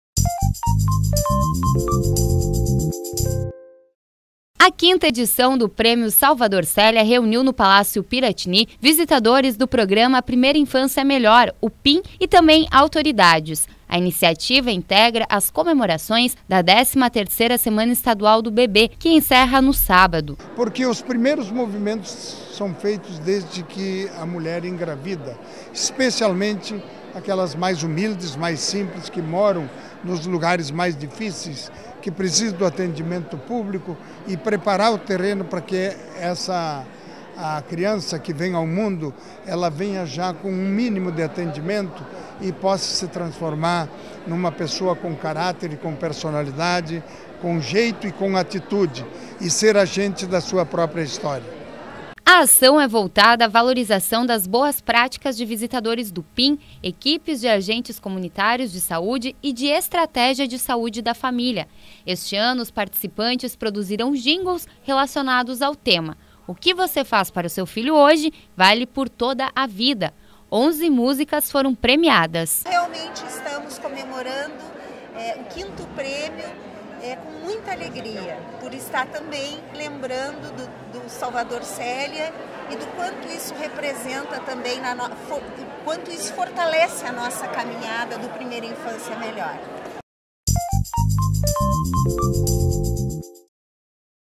Governador participa da entrega do Prêmio Salvador Celia do Programa Primeira Infância Melhor
O governador José Ivo Sartori participou, nesta terça-feira (24), do anúncio dos vencedores da 5ª Edição do Prêmio Salvador Celia, do Programa Primeira Infância Melhor (PIM), uma ação voltada à valorização das boas práticas de visitadores do PIM, Equipes de Agentes Comunitários de Saúde (EACS) e de Estratégia de Saúde da Família (ESF) junto às famílias e comunidades atendidas.